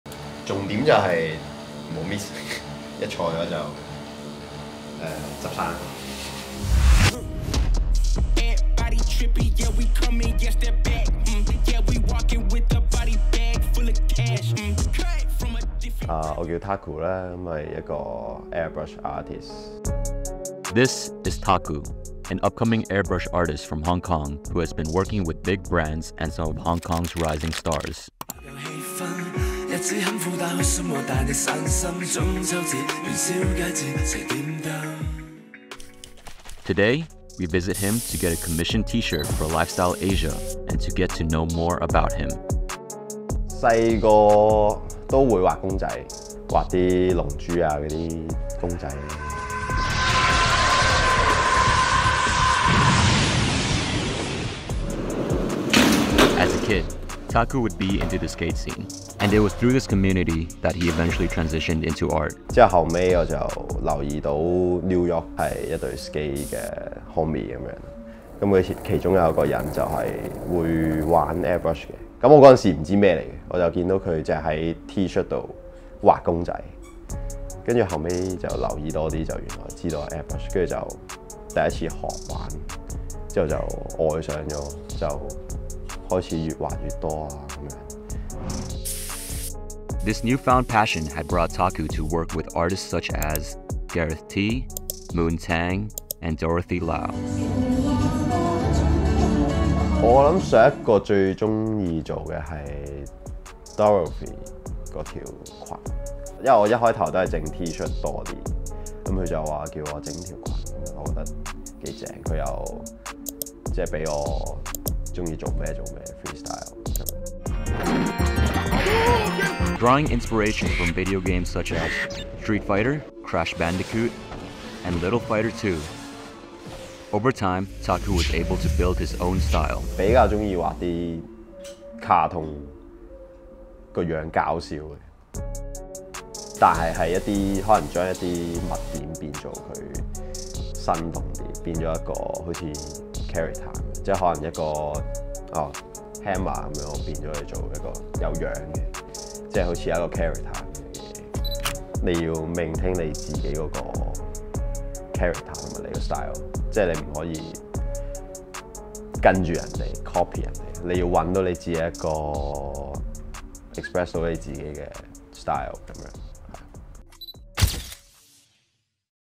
Introducing Taku: Upcoming Airbrush artist working with some of Hong Kong's Rising Stars
Taku is a very talented airbrush artist out of Hong Kong who has been working with stars like Gareth, Moon Tang, Dorothy Lau and brands like Casetify, Victoria, and much more! We got a commissioned t-shirt by the young artist and got to know more about him. From his skating background, to how cartoons and video games influenced him, we got the full scoop in how Taku became Taku.

Credits:
Video Director: Ryan Putranto
Produced by: Ambrose Leung
Translation: Astor Chan and Ryan Putranto